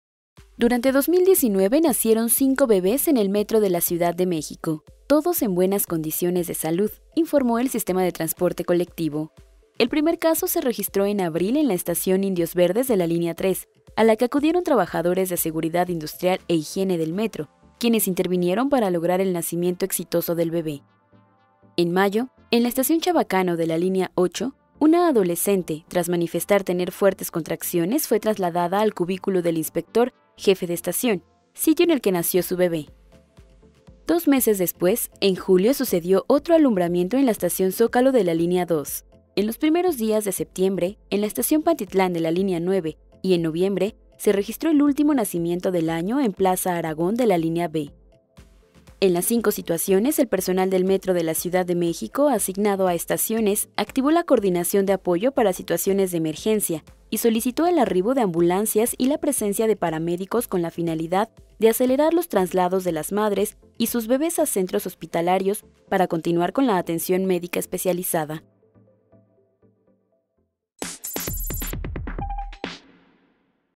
En 2019 nacieron cinco bebés en el Metro
Durante 2019 nacieron cinco bebés en el Metro de la Ciudad de la Ciudad de México, todos en buenas condiciones de salud, informó el Sistema de Transporte Colectivo.